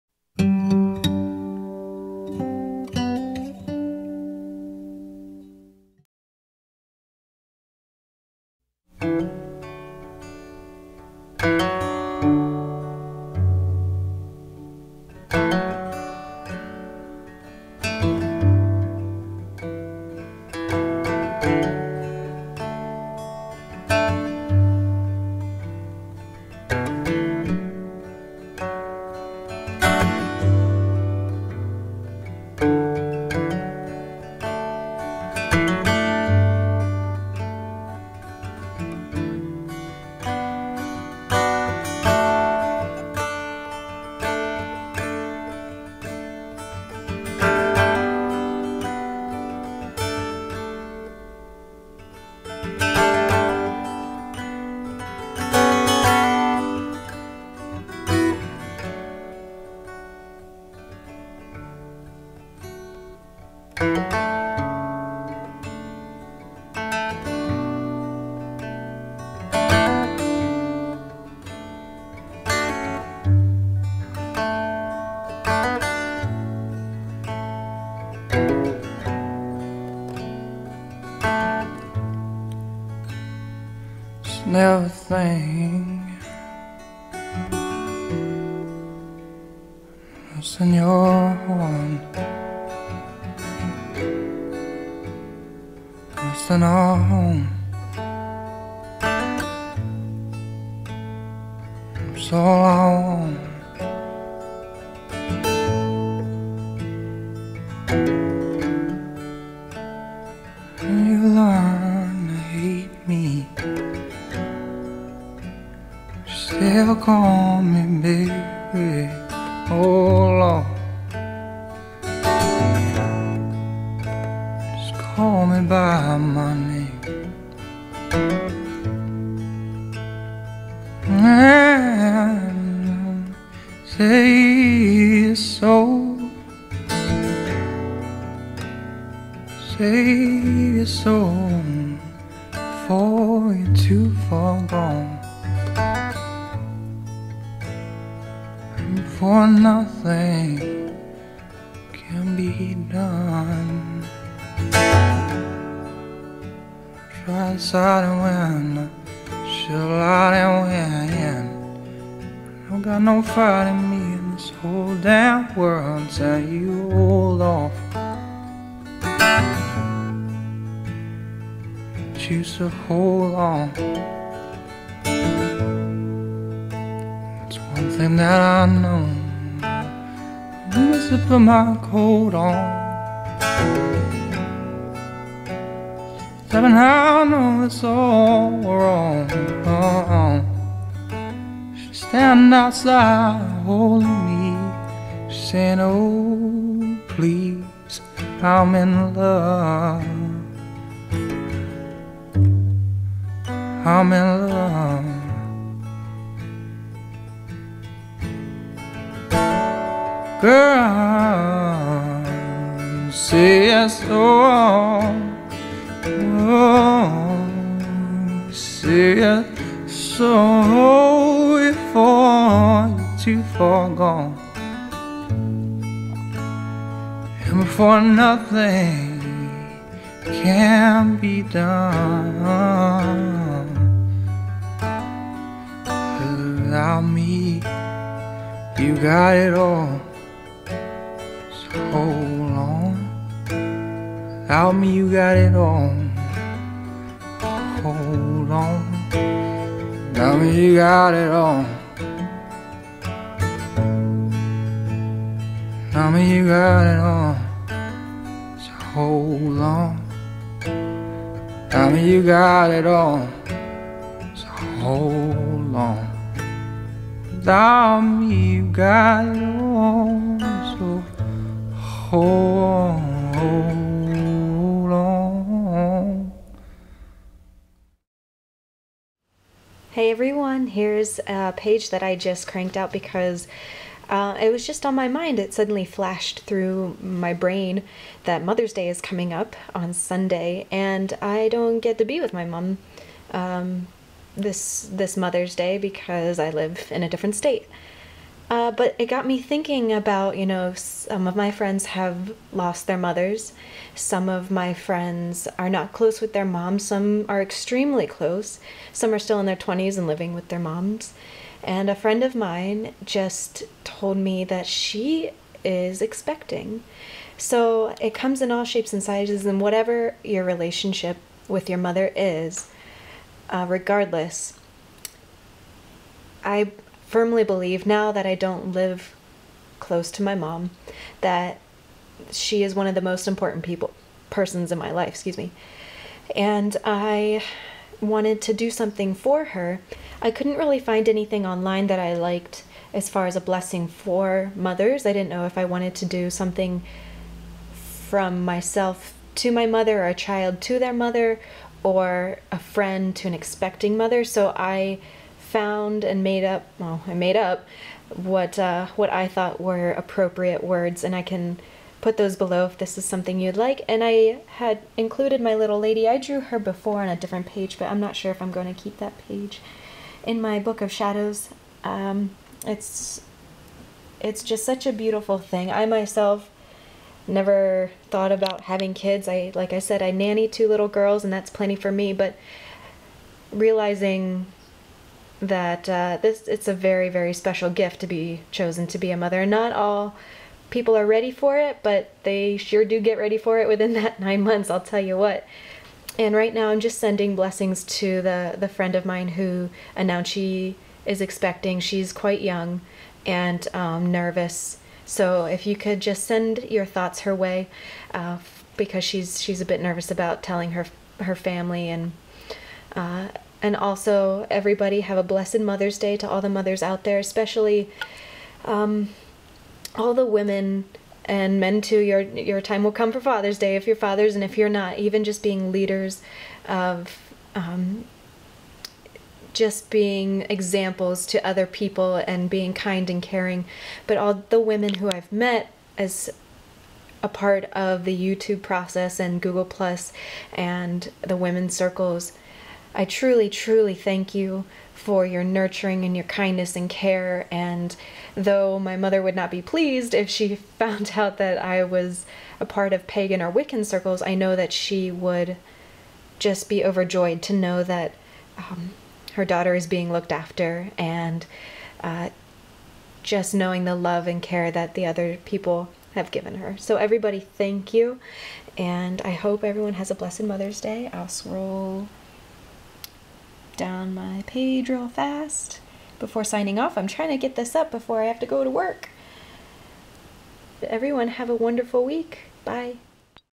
A Blessing for Mothers : Book of Shadows
Mothers day is just around the corner. Created a book of shadows pages to honor all those amazing women. BUT ALSO, to all those who lead by loving examples and nurturing hearts, male or female many blessing unto you.

~~~{ May your grace and wisdom live evergreen in the hearts of your children. May the love you give and sacrifices you make never be forgotten. May I forever cherish your strength and guidance which steadily leads me down life's ever twists and turns. I honor and bless your name and the life's blood you gave unto me that runs fiercely through my veins. I love you. Blessed Be. }